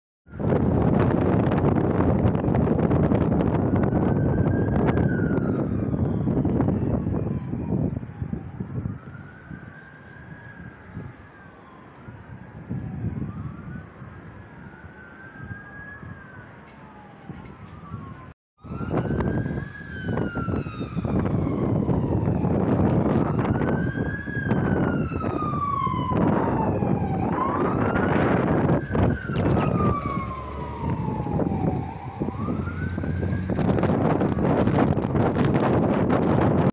Πυρκαγιά στη Λ. Βάρης - Κορωπίου
Φωτιά σε χορτολιβαδική έκταση, εκδηλώθηκε λίγο μετά τη μία σήμερα το μεσημέρι στο Κορωπί, κοντά στη Λ. Βάρης - Κορωπίου.